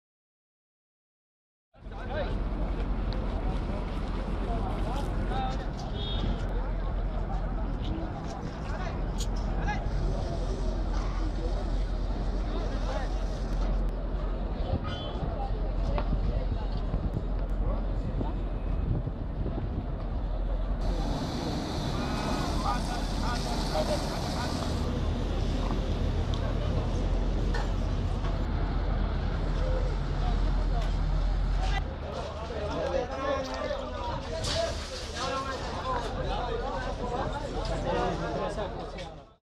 Car bomb kills 15 at busy Baghdad intersection
A car bomb ripped through a crowded Baghdad intersection during morning rush hour on Tuesday, killing 15 people, Iraqi security officials said. Duration: 00:39